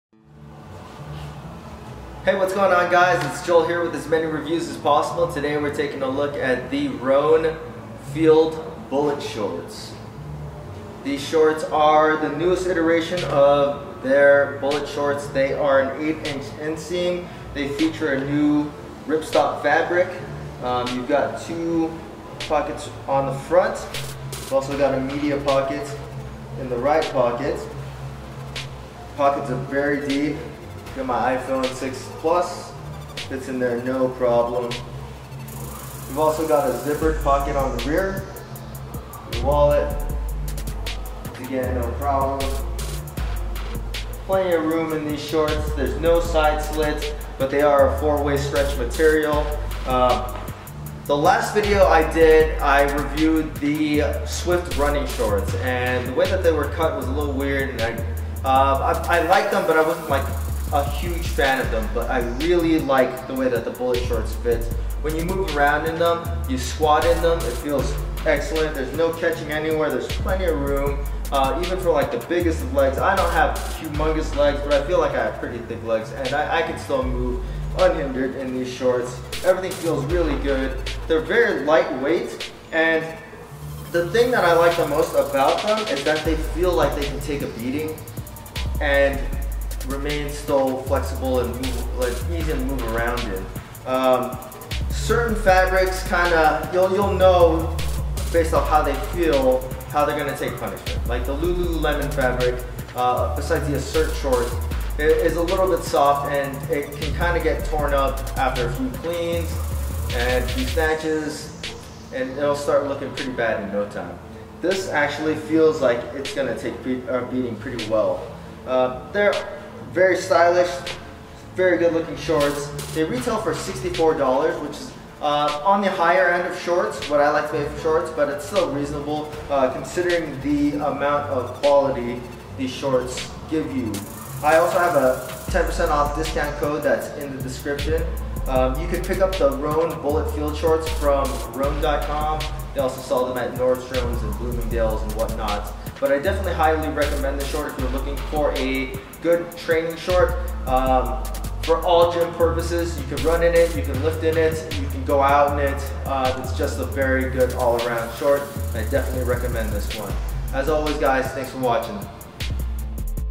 Rhone Field Bullitt Shorts Review - Best Mens Activewear
I love the quality of Rhone clothing, and the Bullitt shorts are my favorite piece that's come from them as of yet!

Code AMRAP10 for 10% Rhone.com